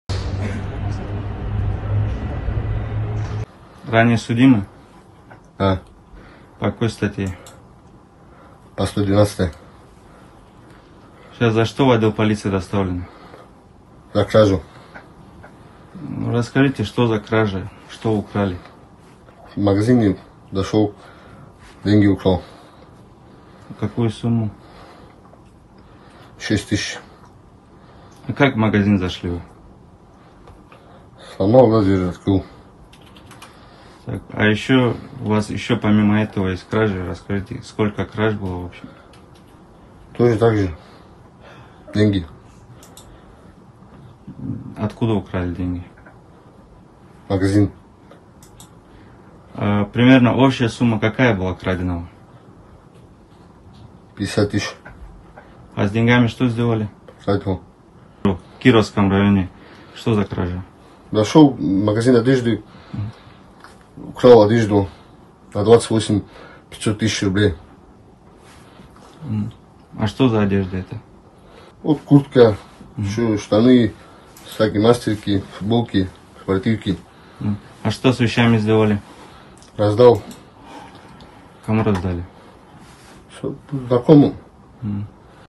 В Дагестане задержали серийного грабителя
Житель села Новочуртах Новолакского района задержан сотрудниками полиции по подозрению в совершении на территории Дагестана серии краж, сообщили 27 апреля в пресс-службе главного управления МВД по республике.

Установлено, что 25-летний мужчина путем взлома проникал в магазины и кафе, похищал деньги и имущество. В настоящее время установлена его причастность к четырем эпизодам краж.

«Так, в одном из случаев злоумышленник в период времени с 3:30 до 4:00 в Махачкале проник в магазин, расположенный в микрорайоне “Пальмира”, по улице Газпромная и совершил кражу денег», – рассказали в МВД региона.

Задержанный признал свою вину. Так, награбленное в одном из магазинов одежды он, по его словам, раздал знакомым.